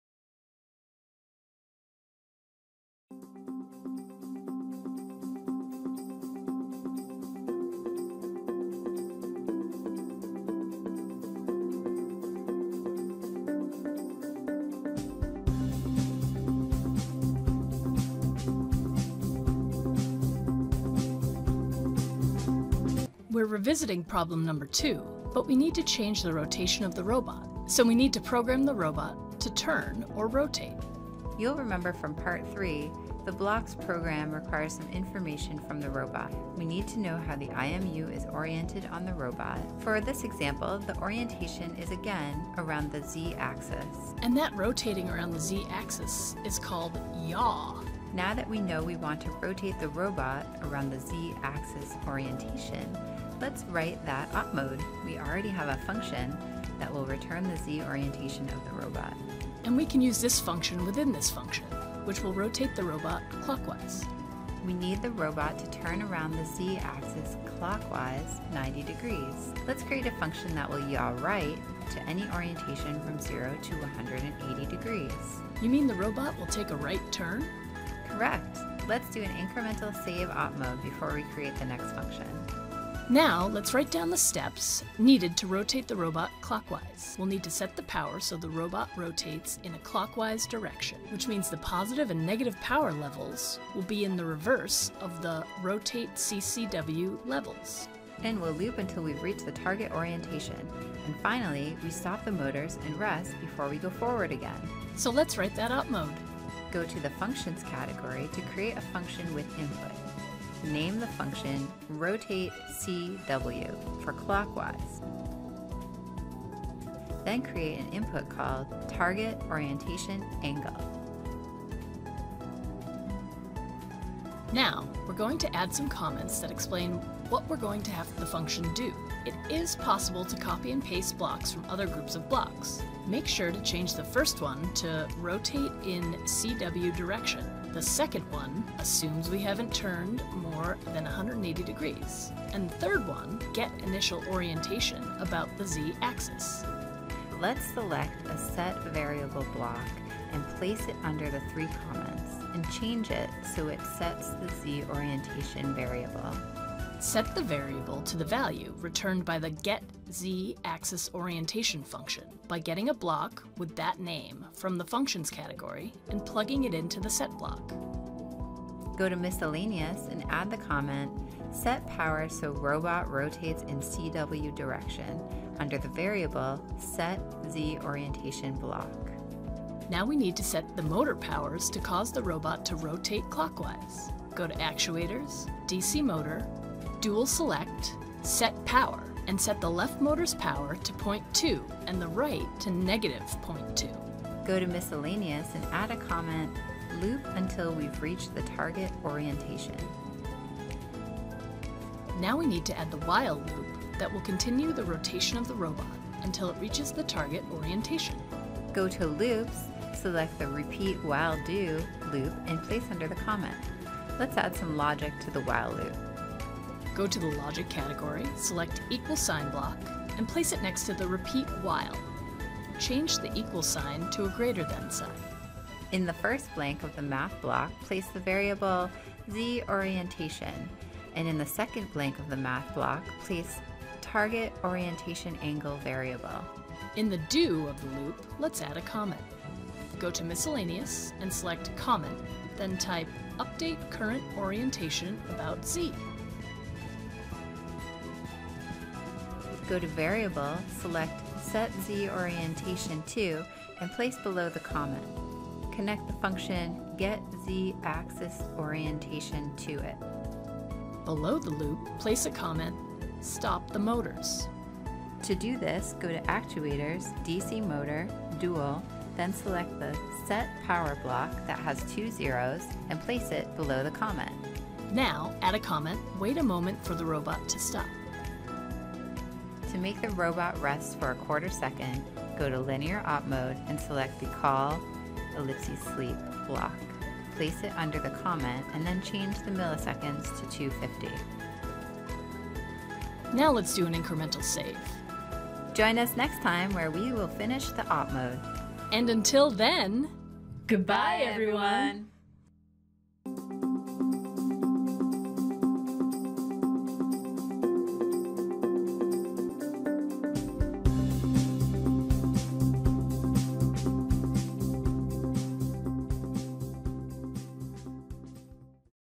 Part 4 - Rotate Clockwise
Create a function for the robot to turn right.